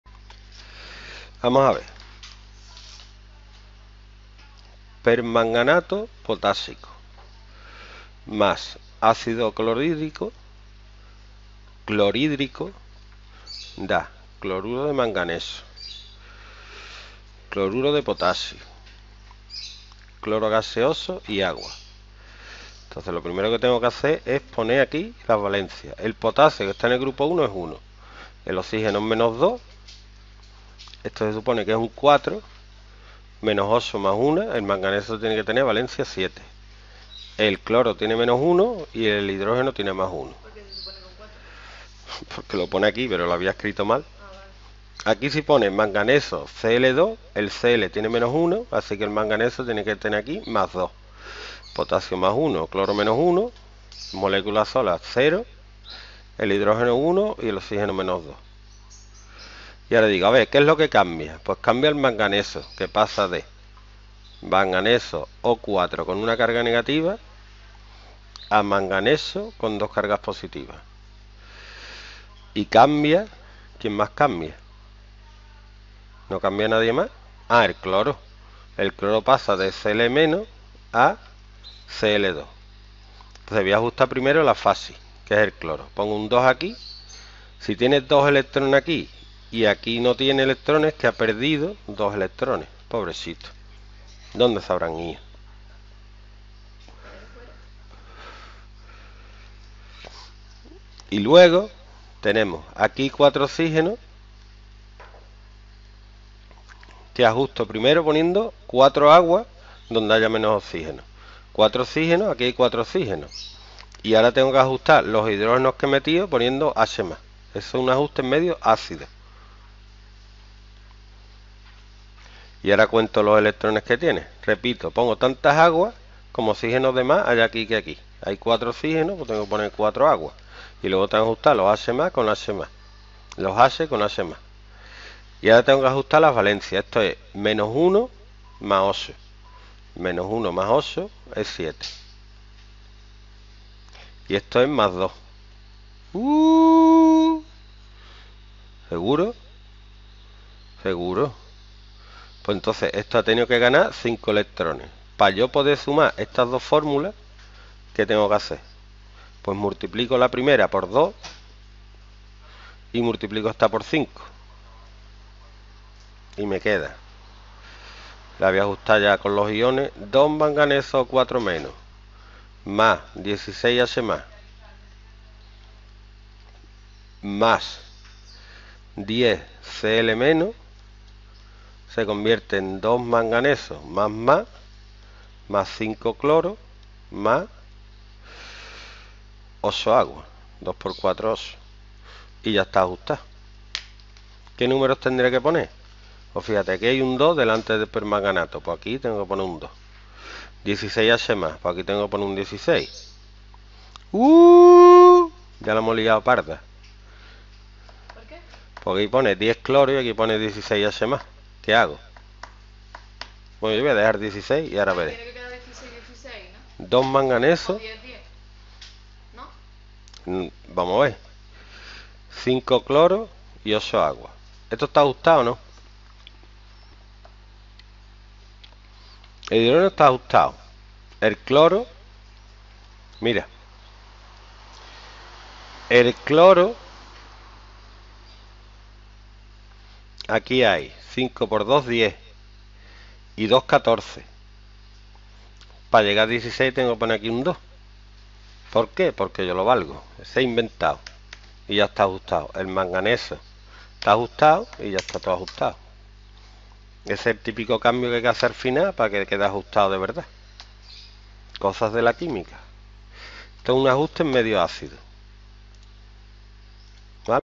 Ajuste de una reacción redox en medio ácido Química 2º Bachillerato Academia Usero Estepona
Ajustamos la reacción del permanganato potásico con ácido clorhídrico en medio ácido. Es una reacción redox en la que hemos de seguir los siguientes pasos:
1) Poner los números de oxidación.
2) Escribir la ecuación del elemento que se oxida y del que se reduce.
3) Ajustar el número de oxígenos añadiendo moléculas de agua donde existan menos oxígenos.
4) Ajustar el número de hidrógenos colocando H+ donde existan menos hidrógenos.
5) Ajustar las cargas con electrones.
6) Sumar las dos fórmulas.
7) Llevar los coeficientes a la ecuación original.
Oxidación y reducción
Química 2º Bachillerato